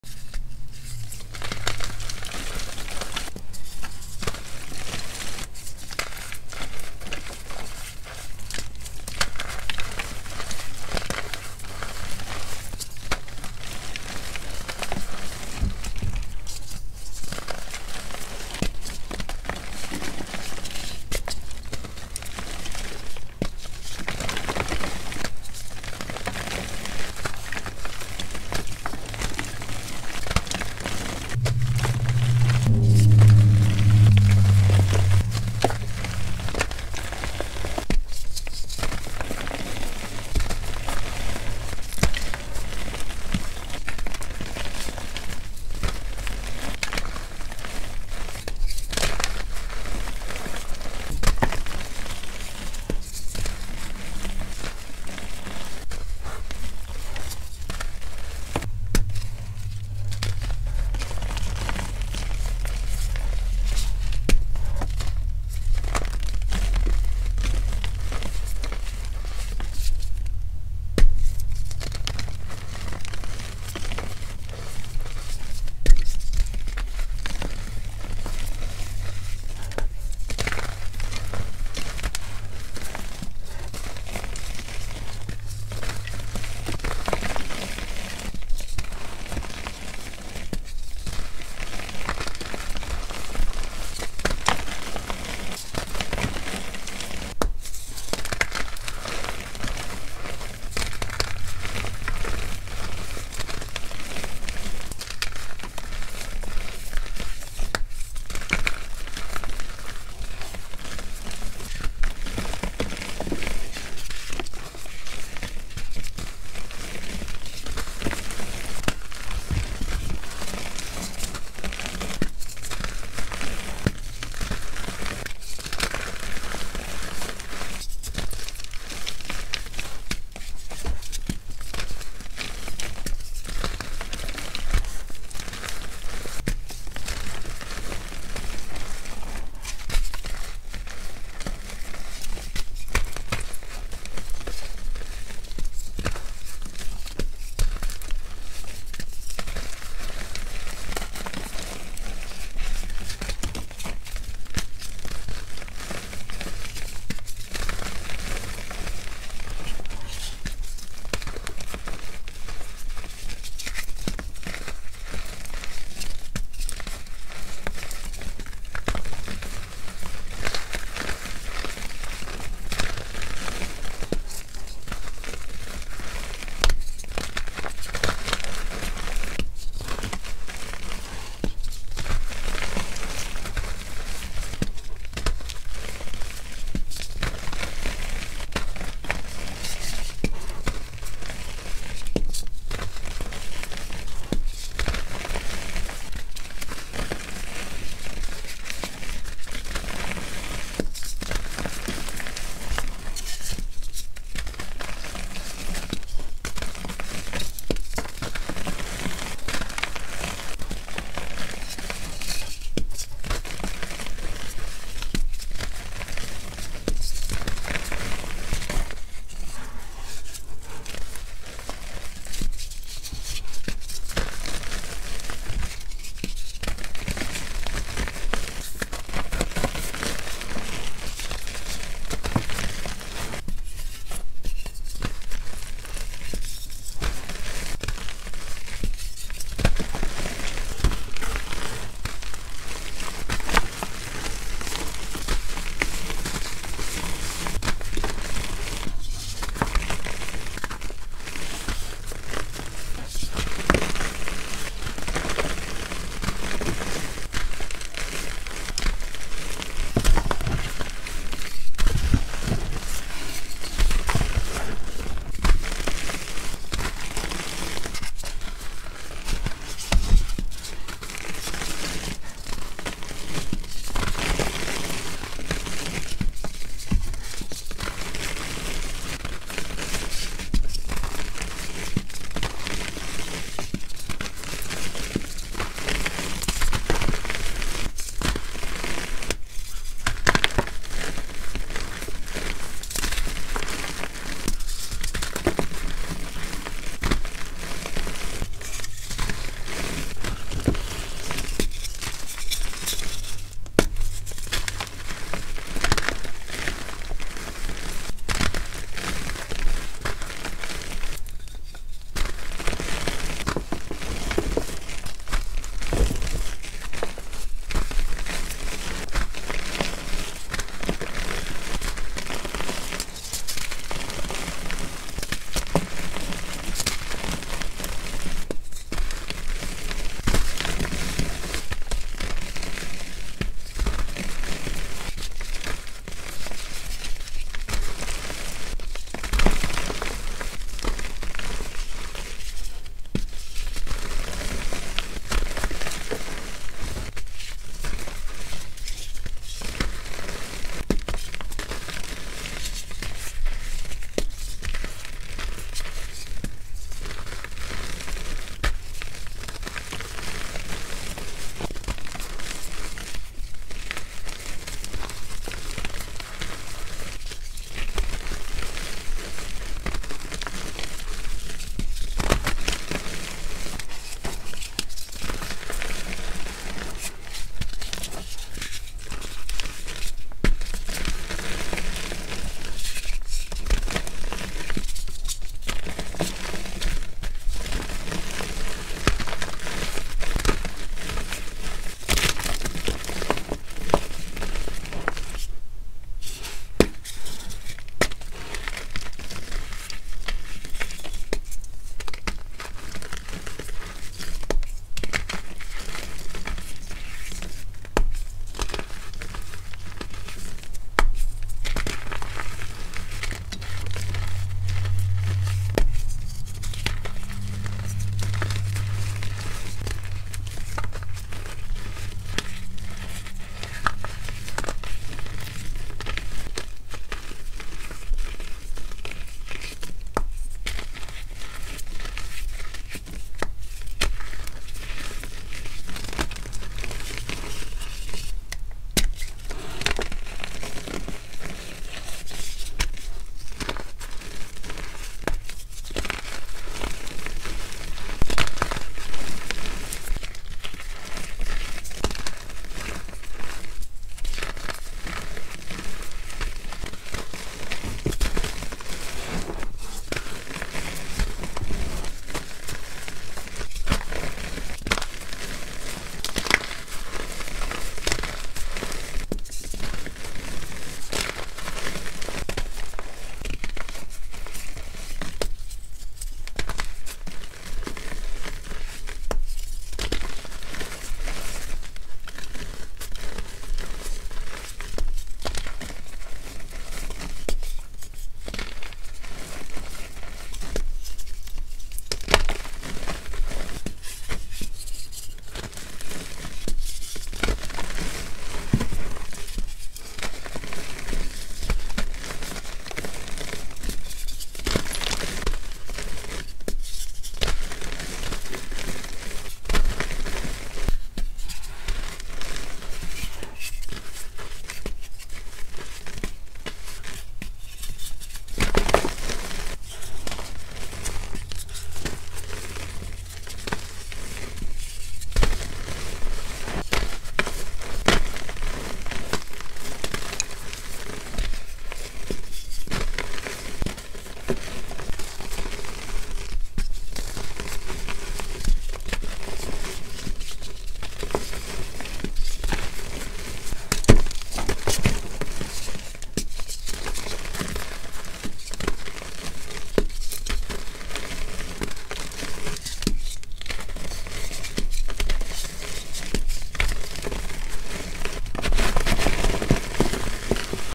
New Years Eve mass crush | REGULAR EDIT
I didn’t get this added into the massive edit video so figured I’d post it separately for those that would like it. I don’t typically like edits all that much (they are good but I rather watch the whole) and I think because I love this crush so much I’m enjoying this edit as well!

Absolutely no posting/editing of my content on any platform!

Thank you for all of your love and support! I appreciate you all so so much.